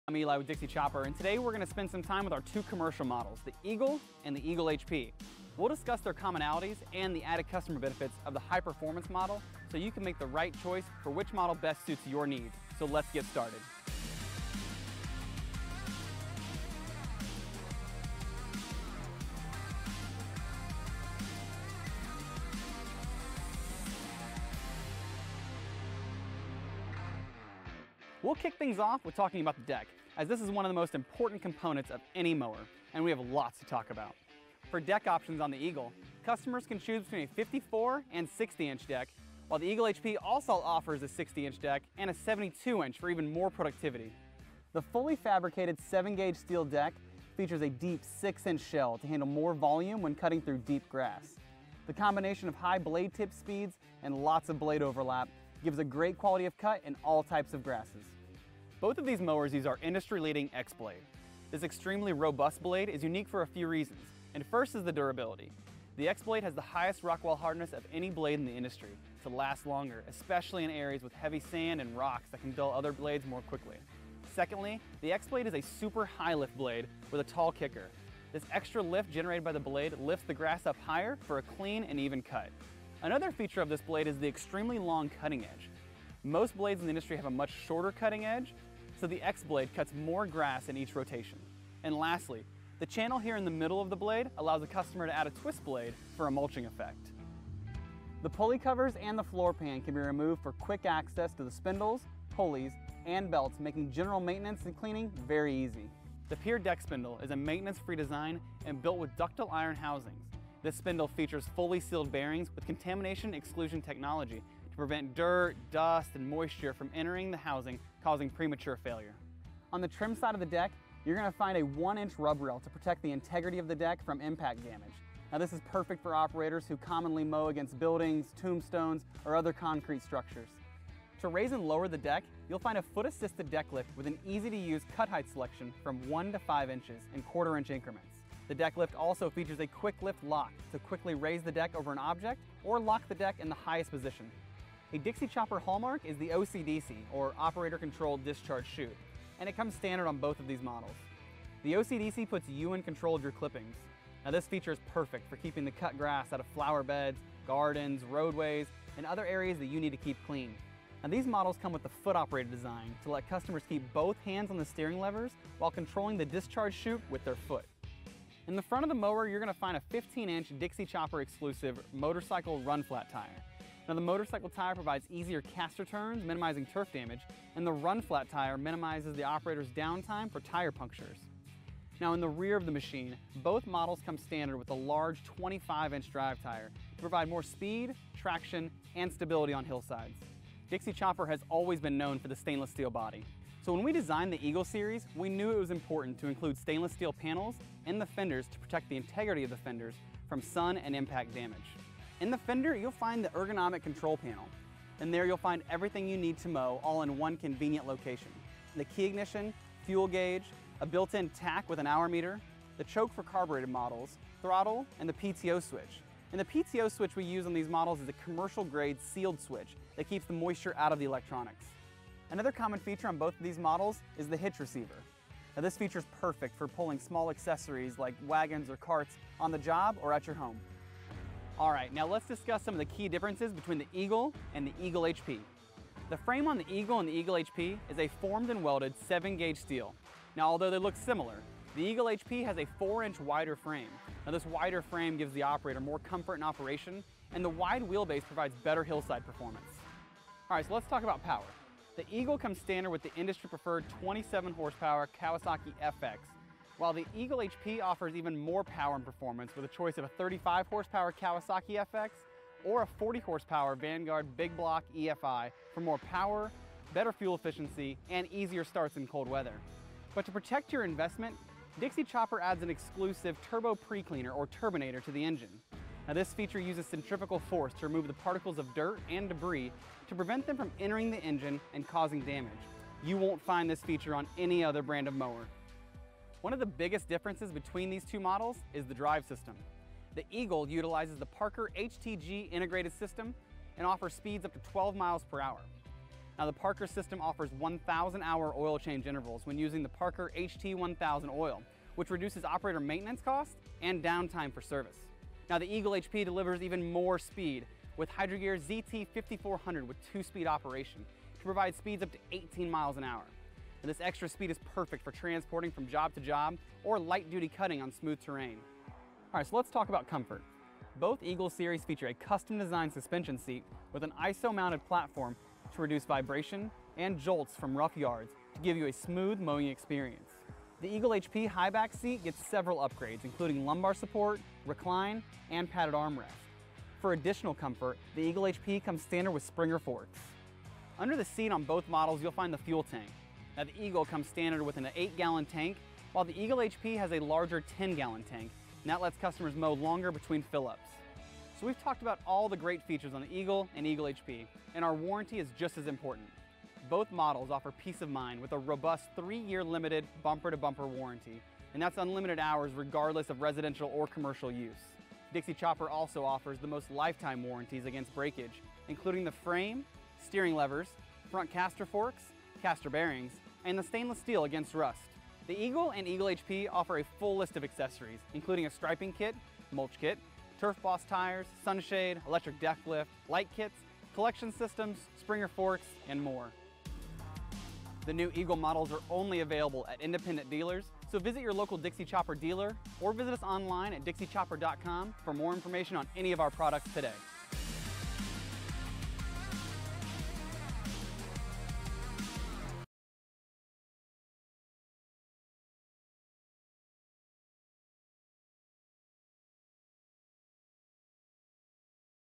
Dixie Chopper Eagle & Eagle HP Walk-Around
The Eagle and Eagle HP are the newest heavy-duty commercial zero-turn mowers from Dixie Chopper. Check out all of the features and benefits of these two models and if the Eagle HP is worth the upgrade for your large property or commercial mowing needs.